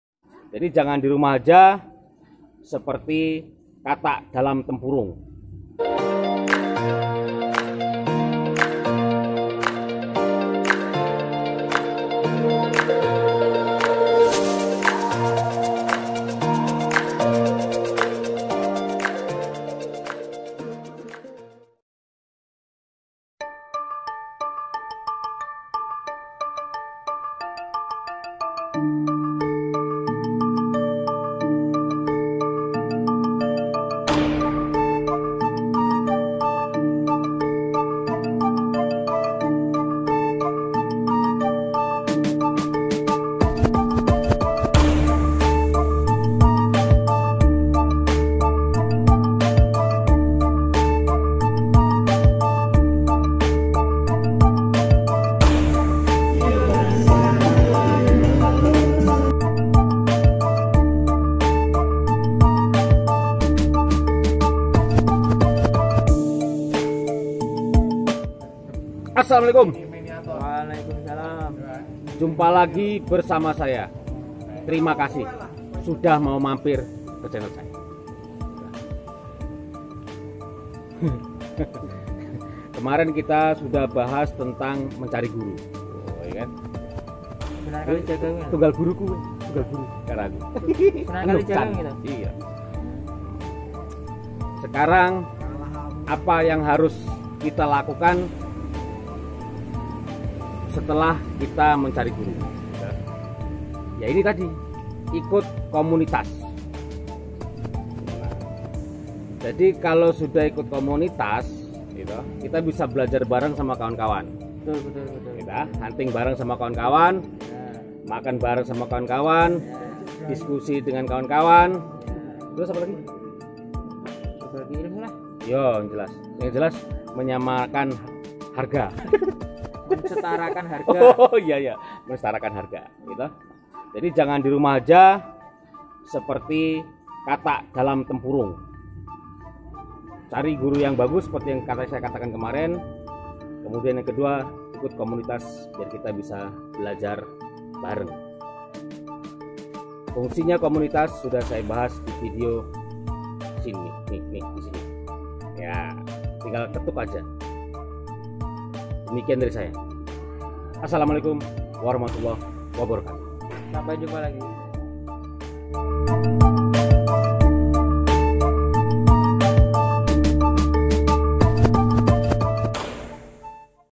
Tips Buat Photographer pemula 2| Wajib ikut komunitas
Setelah diepisode sebelumnya kita membahas tentang tips bagi pendatang baru di dunia photography yaitu " MENCARI GURU ", Nah kali ini kita akan bahas tentang photographer baru harus ikut komunitas (.)